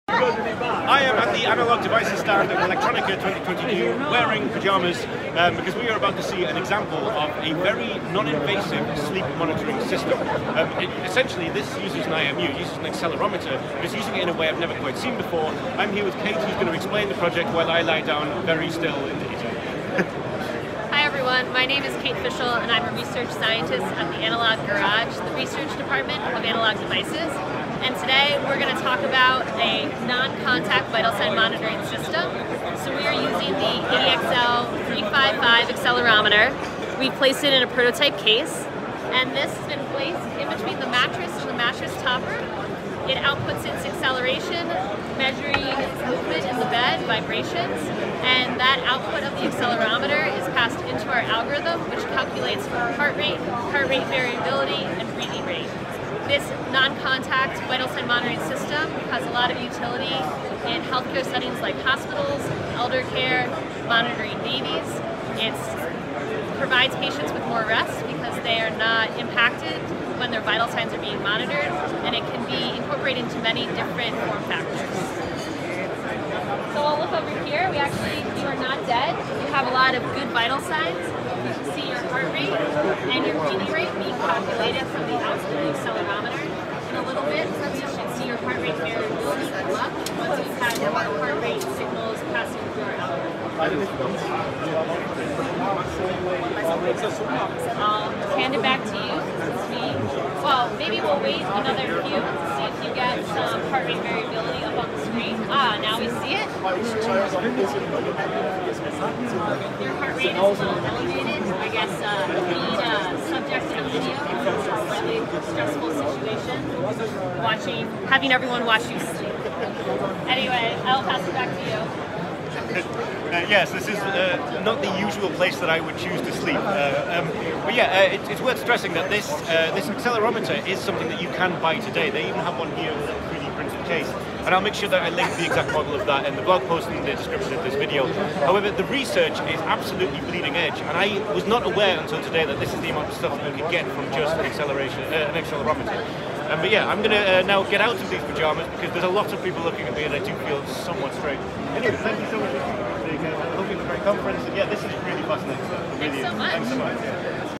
Contactless Heart and Breathing Tracking with a ADXL355 Accelerometer
Non-invasive healthcare monitoring is a big theme in modern smart medical device development. Alongside adding connectivity and edge processing to already existing medical equipment, advances in low-power precision sensing allow for new approaches to medical monitoring.

At the Analog Devices stand, two demos caught our eye. That, and the amazing pajamas they let us borrow for the demonstration!

While it was very difficult to lie still and simulate sleeping on a busy conference floor with an audience (though the pajamas did help), the demo was really impressive. During the demo, they were able to get heart rate readings and determine HRV, an important metric that can help determine a variety of potential health problems.

The core challenge of the research project is taking data from the ADXL355 accelerometer, which is a highly precise and low-noise MEMs device, and extrapolating the important signifiers of a sleeping occupant.

It's worth stressing that there wasn't any crazy hardware going on in the bed. The accelerometer is housed in a 3d printed case, and placed between a regular mattress and a topper. The magic, as is so often the case, is in the algorithm Kate and others in the Analog Garage research team have been perfecting.

While the research is still bleeding edge and not ready for public use, the ADXL355 is available in package and in evaluation form factors from Analog Devices.

▬ Support us! ▬▬▬▬▬▬▬▬▬▬